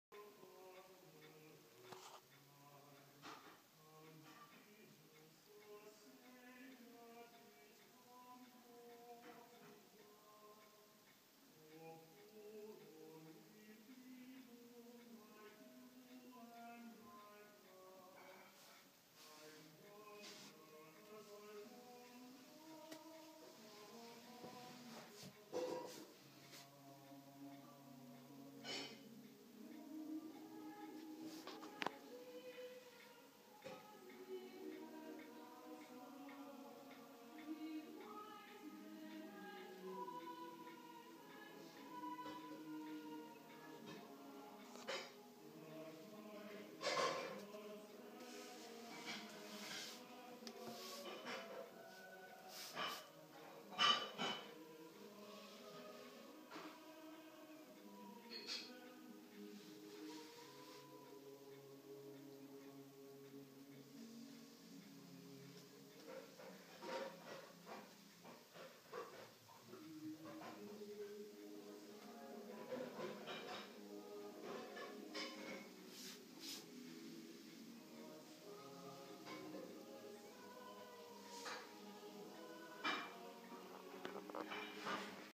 Manger Scene 2012
The latest iteration of the Eiswerth household crèche.